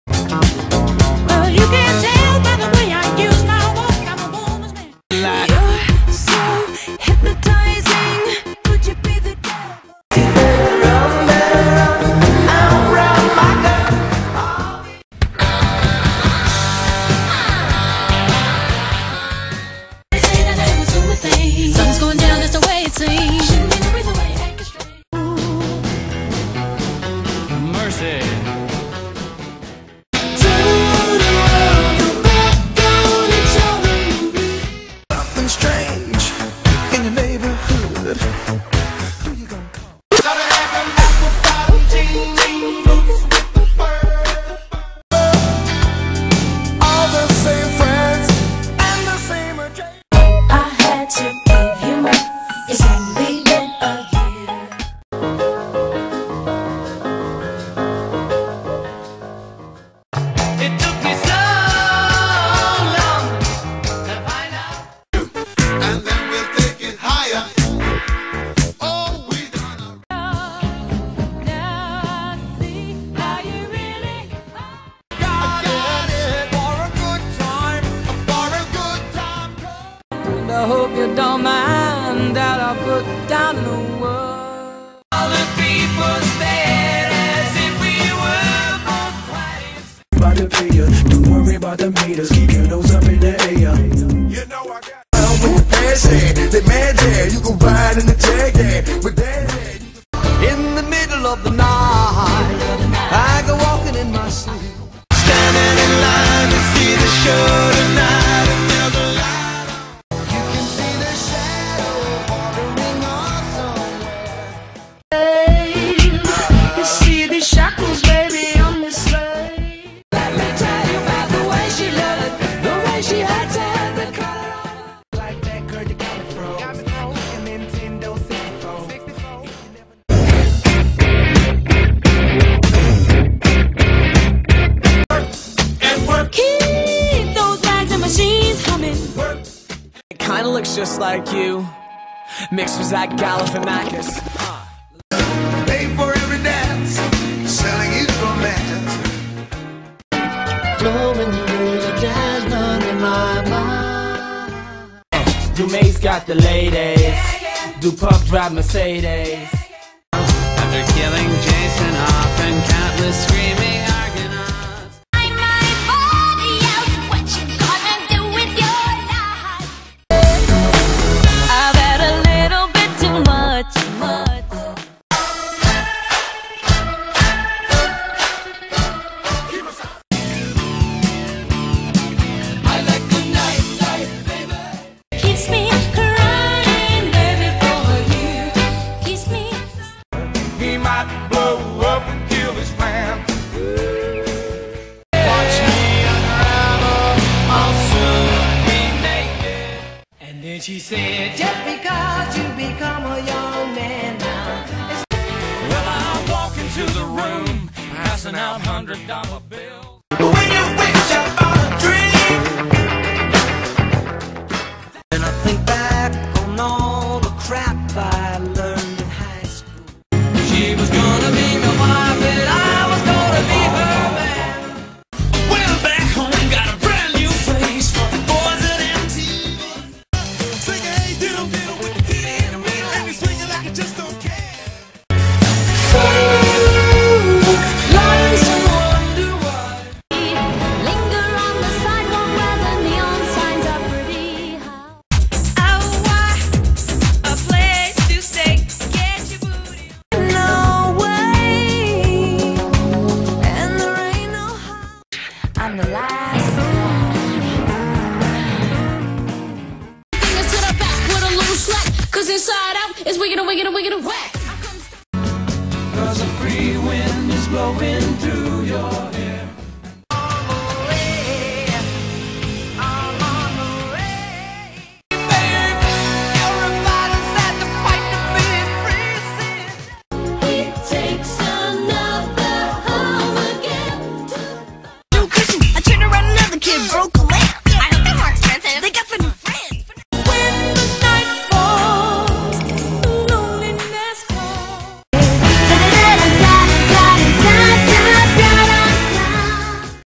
Unthemed Music Clip Sporcle Quiz 9
60 songs, 5 seconds each, lots of fun.